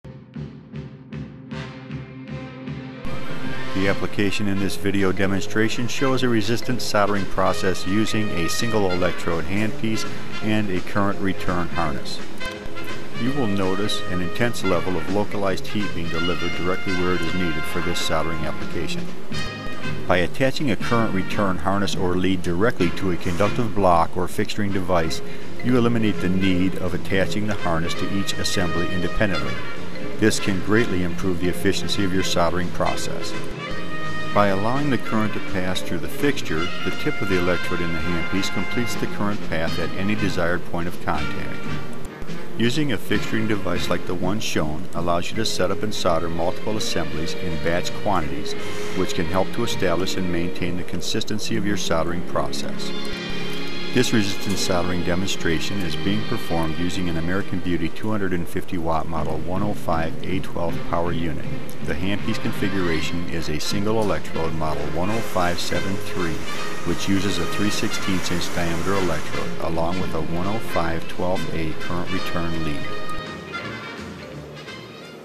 Soldering custom terminal assemblies together with resistance soldering equipment.
This video demonstrates the ability to resistance solder a custom terminal assembly together using a single carbon electrode handpiece and a return harness attached to special fixturing.